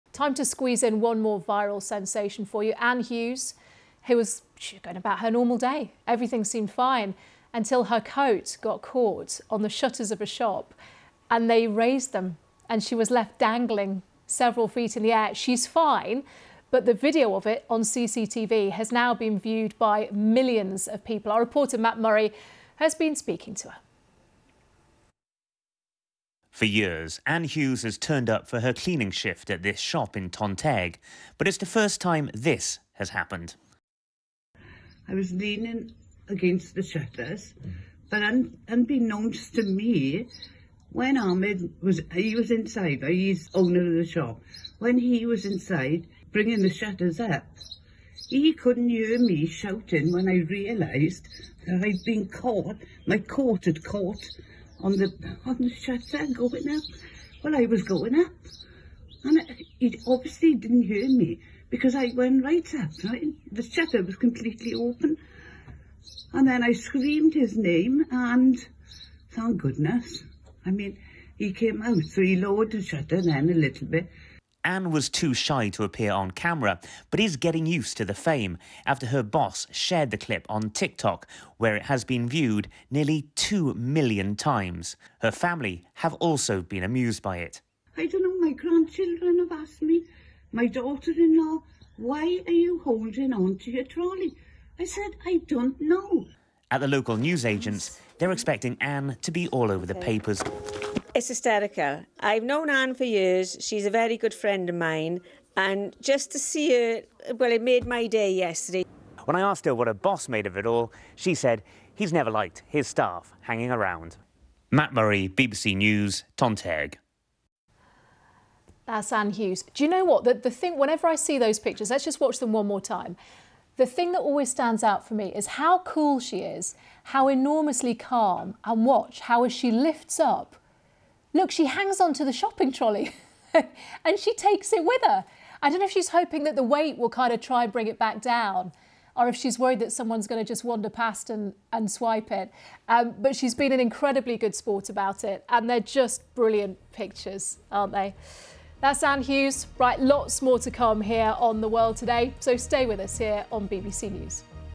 TikTok fame for woman dangling from shop shutters | BBC News
A woman hoisted into the air by shop shutters has joked that she will have to "handle the fame" after a CCTV clip went viral.

Anne Hughes, 72, a cleaner at the Best One convenience store in Tonteg, Rhondda Cynon Taf, was lifted several feet up when her coat got caught.

The shop shared the clip on TikTok, where it's been seen 1.9 million times.

"I'll never hear the end of it," she said. "My boss has said he's never liked his staff hanging around."